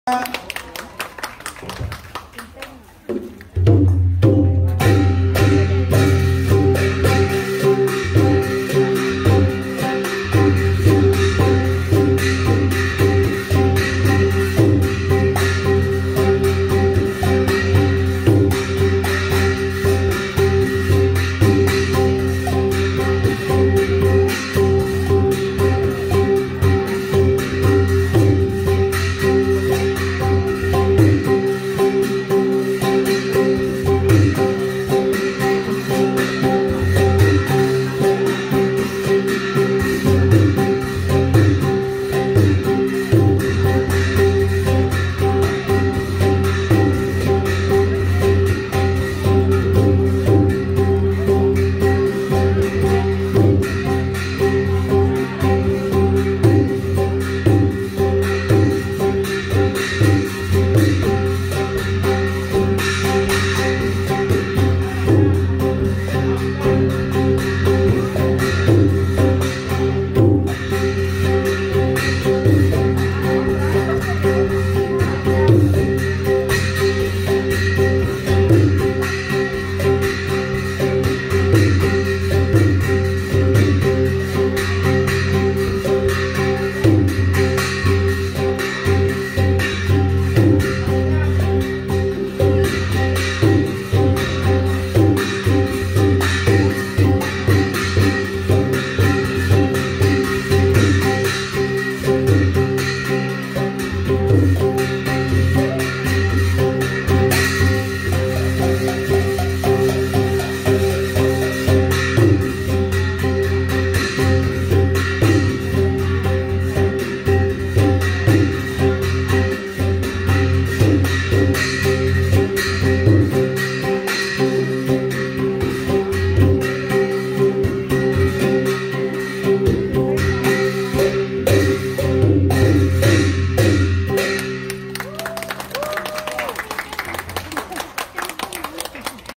ฟ้อนไม้ค้อน
สอนโดยครูนิก ศรัณ  สุวรรณโชติ
พิธีรับใบป่าว โฮงเฮียนสืบสานภูมิปัญญาล้านนา